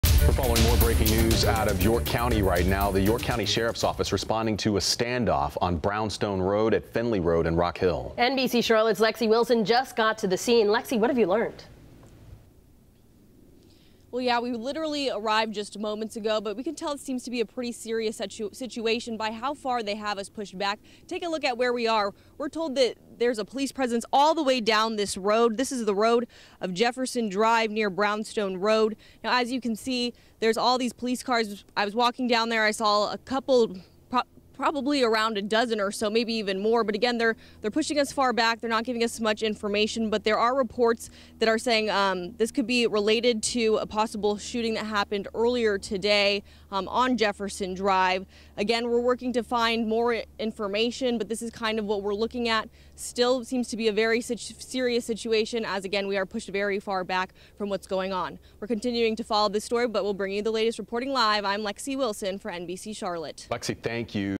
Deputies respond to SWAT situation in Rock Hill
It happened around 3:30 p.m. Thursday on Brownstone Rd. and Finley Rd.